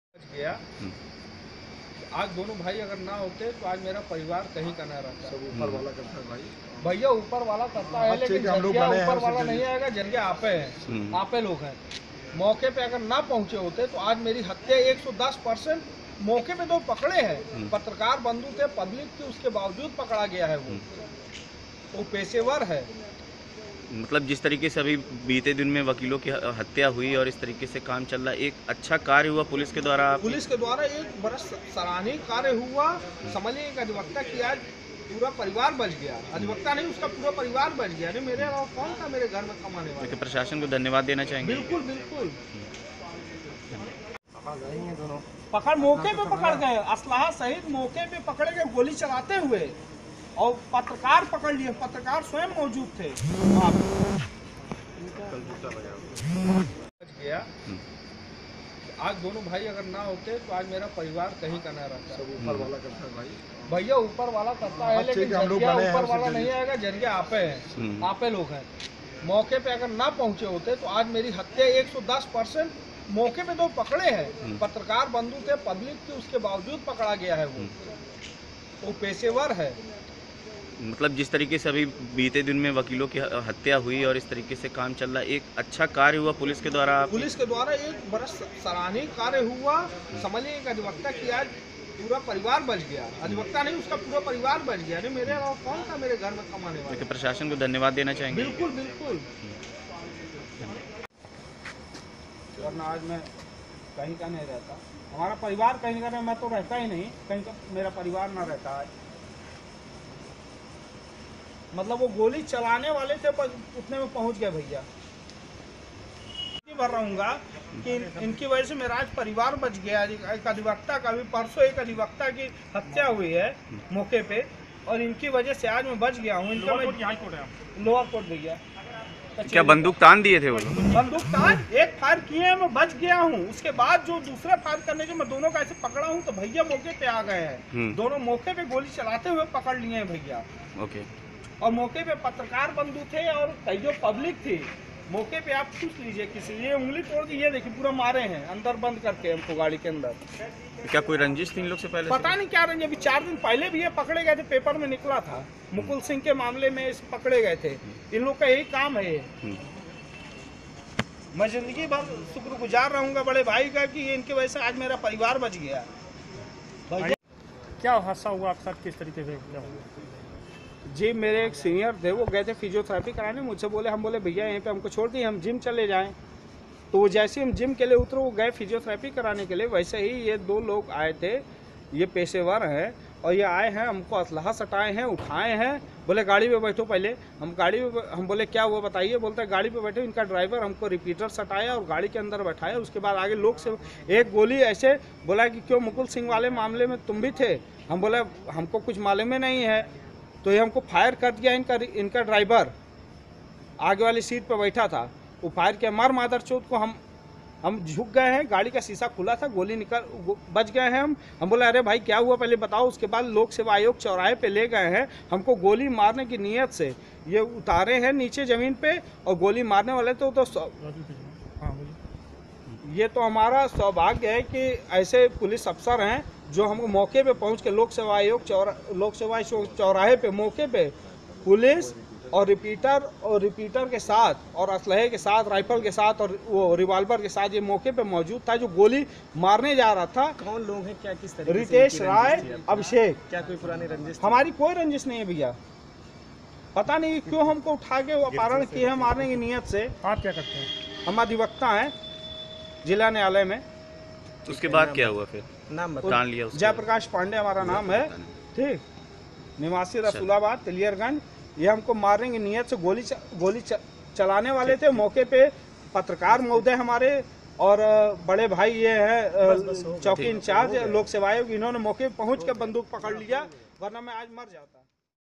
प्रयागराज मे पुलिस की सक्रियता से अधिवक्ता को बचाया गया
प्रयागराज में एक वकील को सरेराह अगवा करने की कोशिश की गई,लोक सेवा आयोग चौकी इंचार्ज रवि शर्मा की सक्रियता से अधिवक्ता को बचा लिया गया।
वकील से सुनिए किस तरीके से प्रयागराज पुलिस प्रशासन ने उसकी जान बचाई